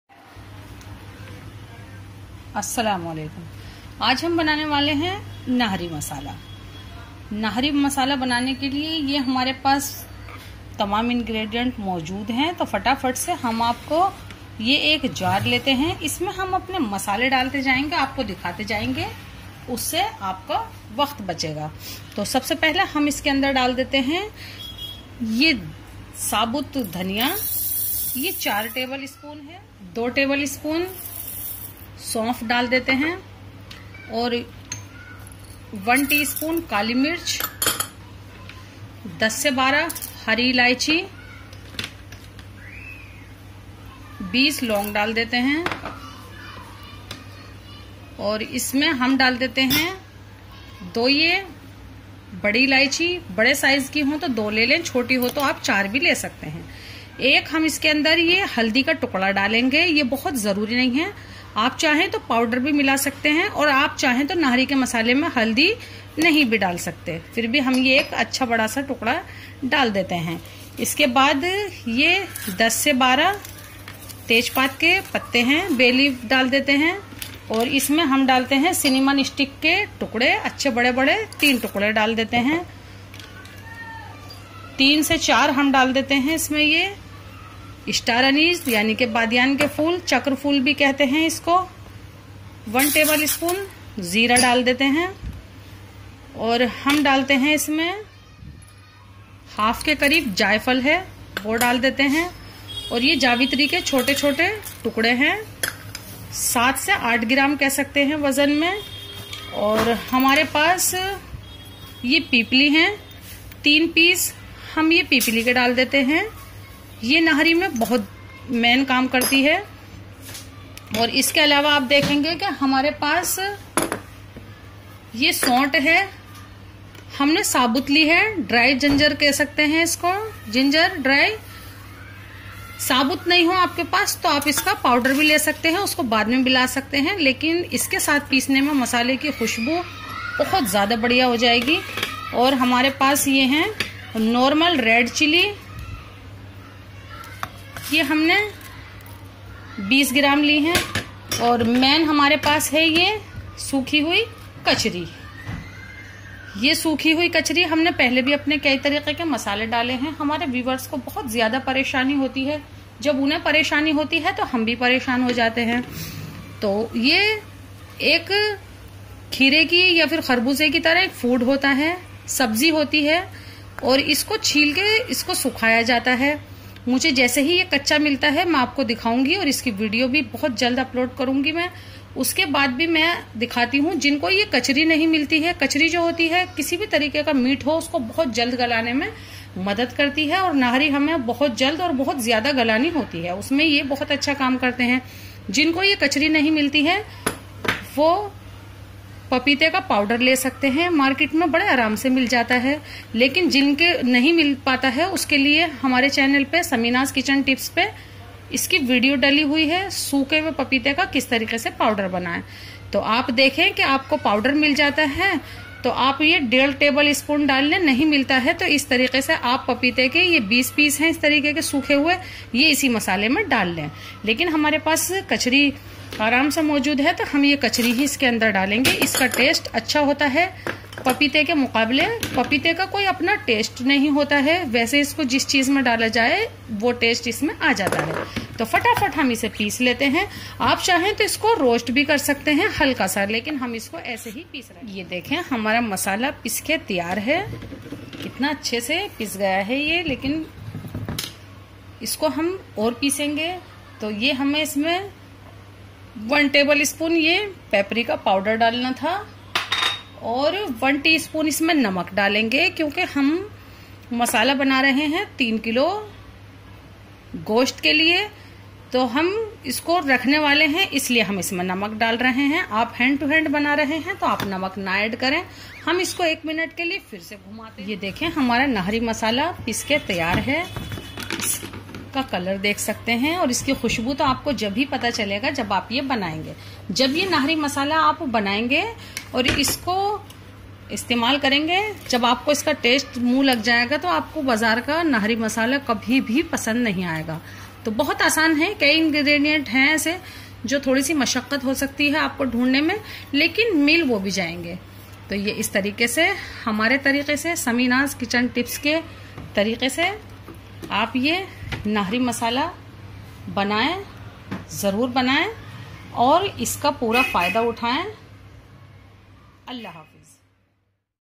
Nihari masala powder | purani Dilli ka famous Nihari masala
4Tbsp Coriander Seeds
2 Tbsp Anice
1Tsp Black Pepper
10 - 12 Green Cardamom
20 Cloves
2 Black Cardamom
1 Piece Turmeric
10-12 Bay leaves
3 Peace Cinnamon Stick
3-4 Star Anice
1Tbsp Cumin
1/2 Peace Of Nutmeg
8-10 Piece Mace
3 Piece Long Pepper
15 Gm Dry Ginger
20Gm Red Chilli
1.5 Tbsp Kachri/ Papaya Powder
1Tbsp Peprica Powder
1 Tsp Salt